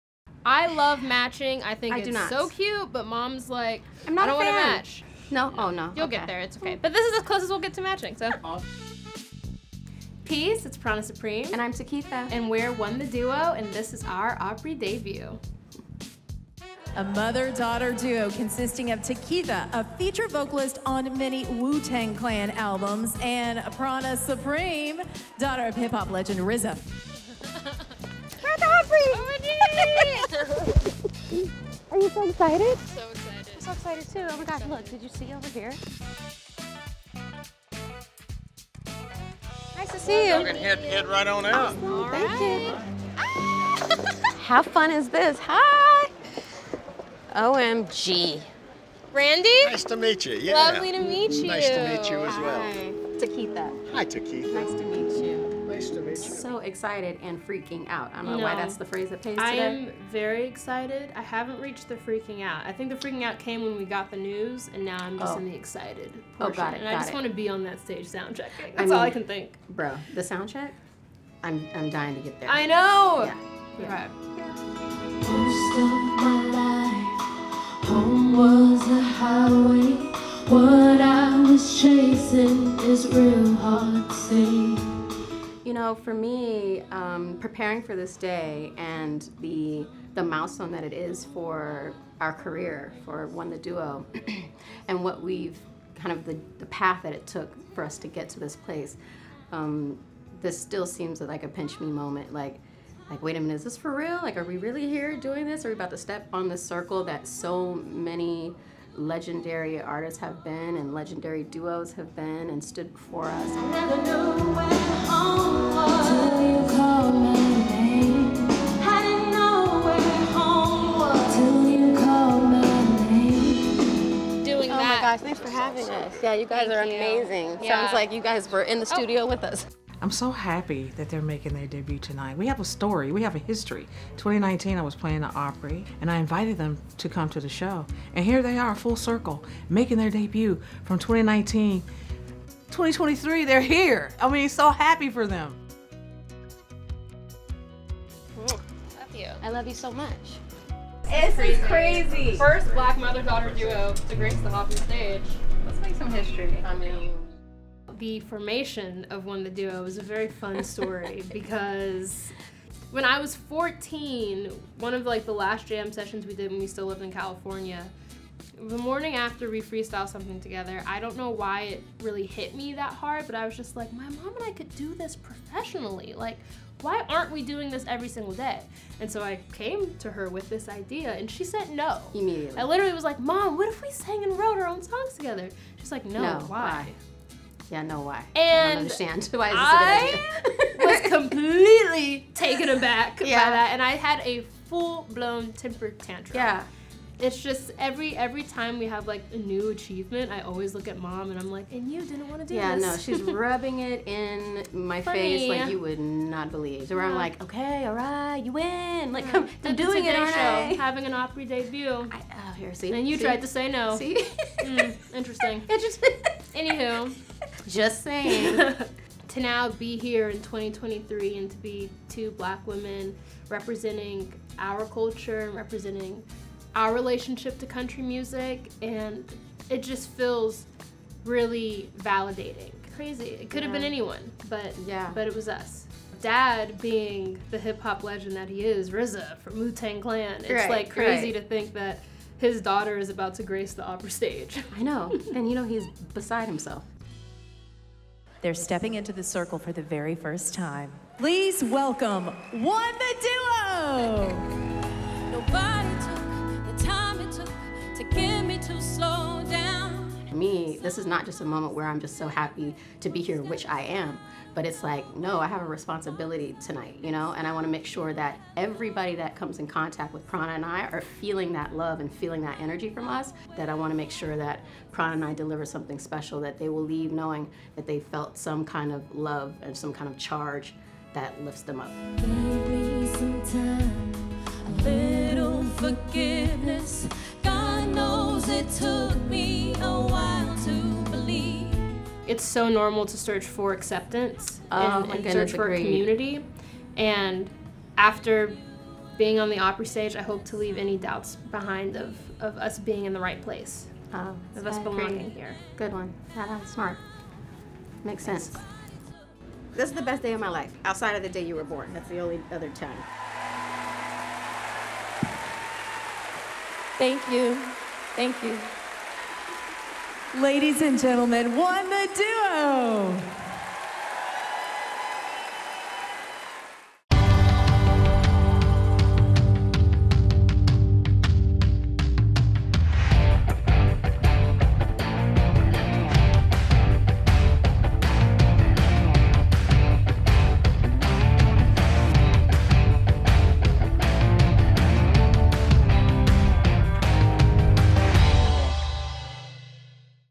O.N.E The Duo | My Opry Debut
Mother-daughter duo – O.N.E The Duo – made history during their Grand Ole Opry debut when they took the stage for the first time. Follow along and go behind the scenes as they play the iconic venue.